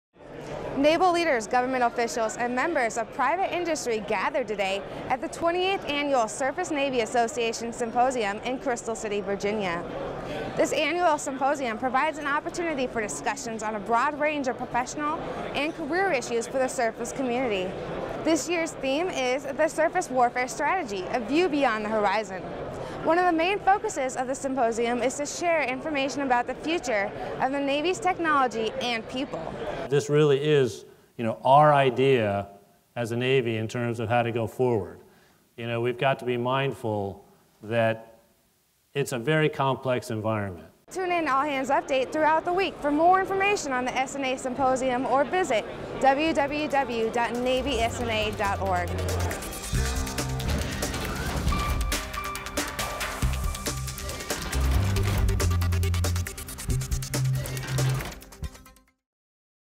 Surface Navy Association Symposium
Naval leaders, government officials and members of private industry gathered to discuss the surface Navy's future at the 28th Annual Surface Navy Association (SNA) Symposium in Crystal City, Jan. 12.